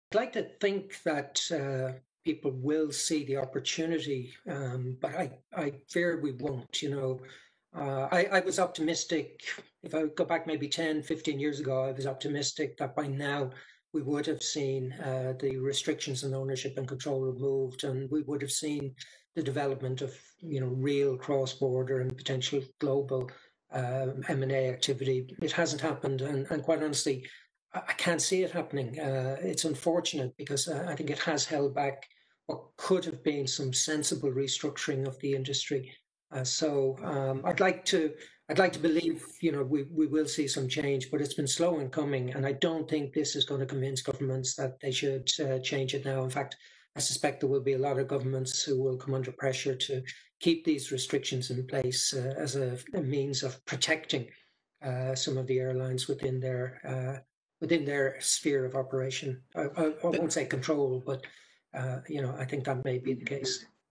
Aviation will restructure slowly
Aviation will restructure slowly rather than quickly. Willie Walsh was interviewed by Andrew Charlton, journalist and MD of Aviation Advocacy.